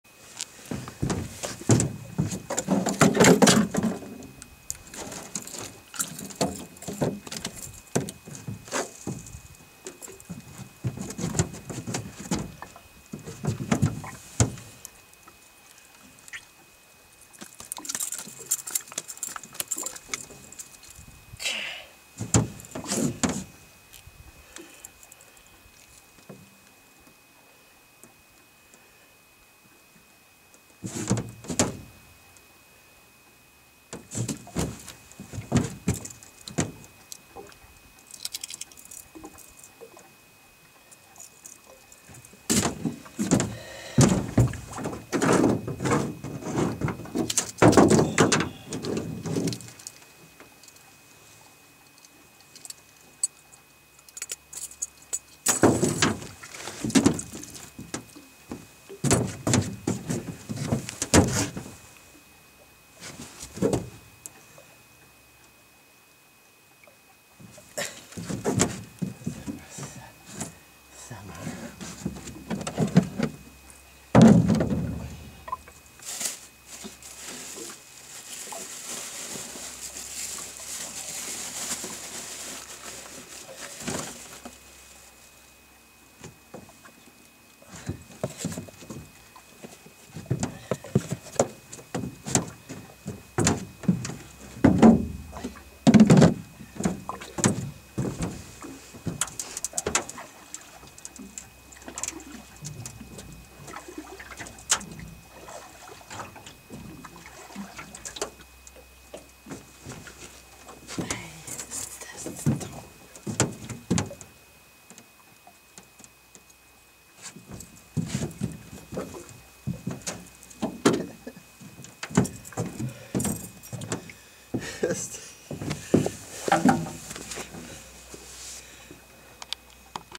lake trout attack this spoon
the erratic action is deadly for trout . use with downriger and  follow the structure at 50 feet . trolling speed is 1.6 to 2 mhp .  lac des trentes et milles , gracefield , quebec , canada . 4 juin 2011 .